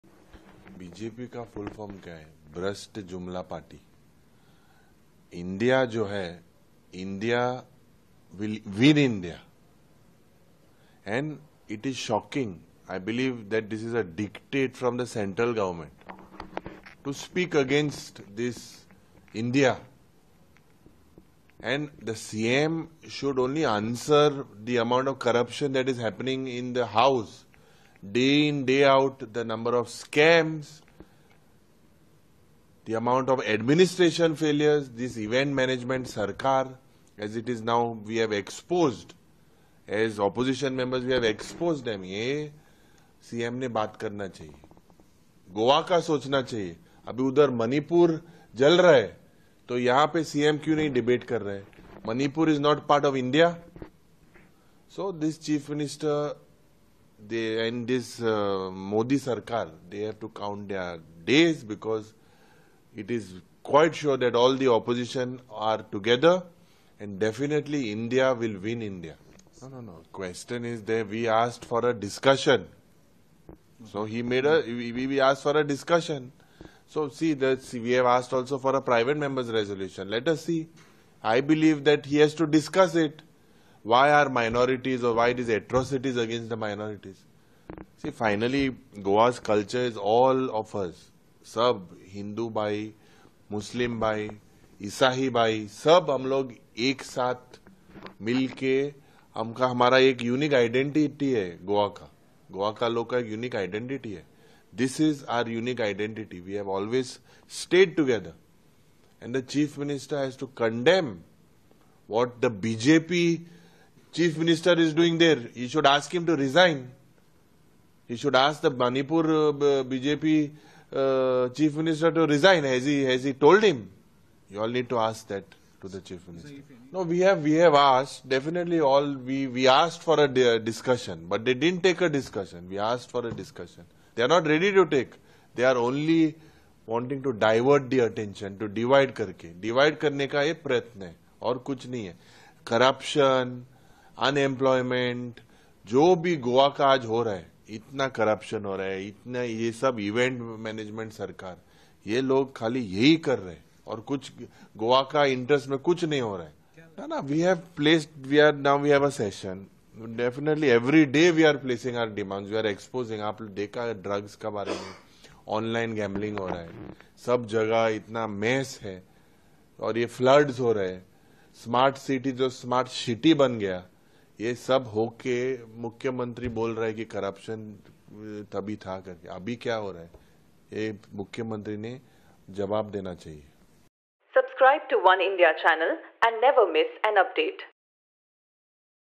Opposition Alliance: Yuri Alemao lashes out at BJP’s dig at INDIA | Watch | Oneindia News
Goa Legislative Assembly Leader of Opposition Yuri Alemao lashed out at BJP’s dig at the Opposition alliance INDIA. Calling it Bhrasht Jumla Party, he said that the Goa CM and the BJP government should start counting their days because I.N.D.I.A will win India.